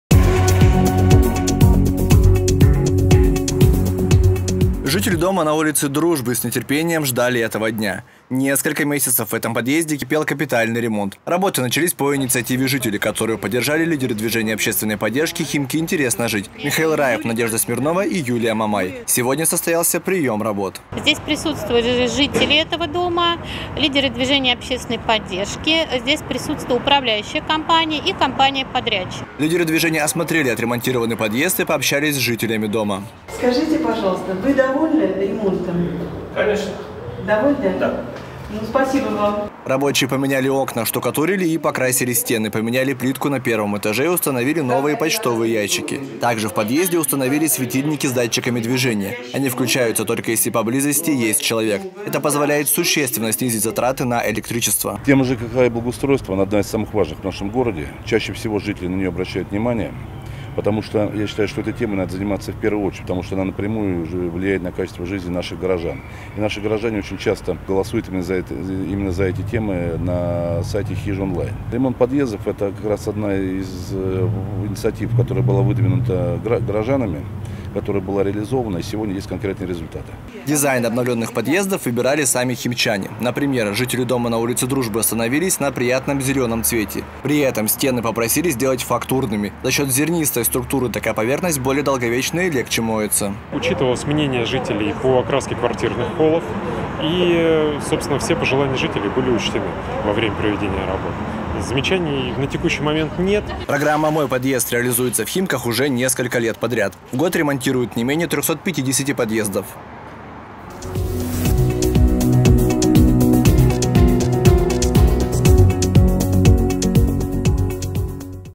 В Химках ремонтируют подъезды. 12.08.2021
Жители дома на улице Дружбы с нетерпением ждали этого дня. Несколько месяцев в этом подъезде кипел капитальный ремонт. Работы начались по инициативе жителей, которую поддержали лидеры Движения общественной поддержки «Химки – интересно жить!» Михаил Раев, Надежда Смирнова и Юлия Мамай.